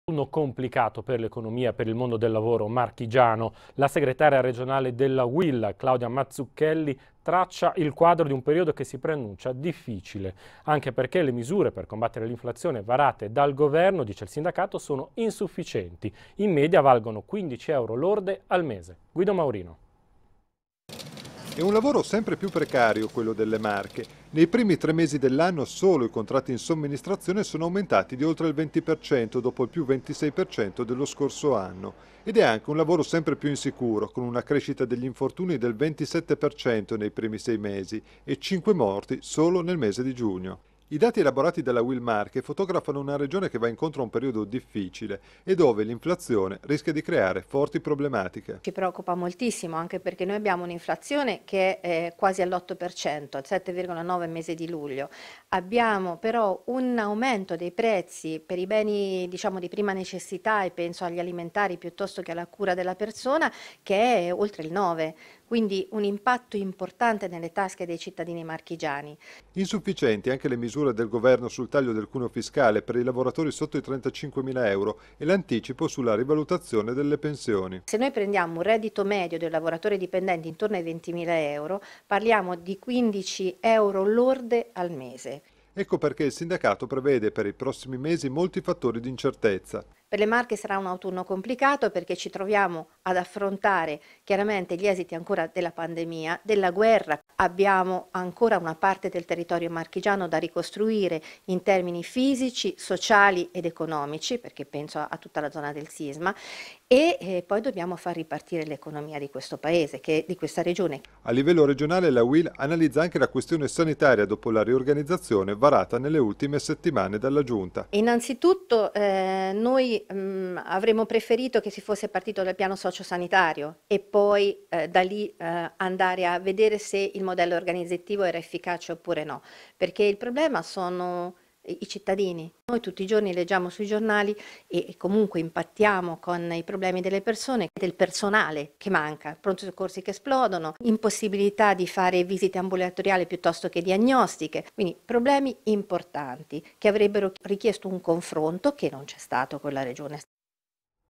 Uil Marche, ristori da poche decine di euro: “Aiuti bis irrilevante”
Il taglio del cuneo fiscale annunciato dal governo per i lavoratori dipendenti rischia di essere irrilevante con poche decine di euro in più in busta paga. I calcoli li ha fatti il Centro Studi della Uil che ha visionato la situazione dei circa 524mila contribuenti marchigiani con un reddito da lavoro dipendente sotto i 35mila euro. “Un lavoratore con un reddito annuo lordo pari a 8mila euro per le mensilità percepite da luglio a dicembre avrebbe un beneficio complessivo di 36,92 euro lordi, circa 6 euro lordi in più al mese – spiegano dalla Uil Marche - Nella fascia di reddito medio dei lavoratori dipendenti del settore privato, pari a circa 20.111 euro annue, il beneficio per il secondo semestre 2022, sarebbe complessivamente di 92,82 euro lordi, 15,47 euro lordi al mese”.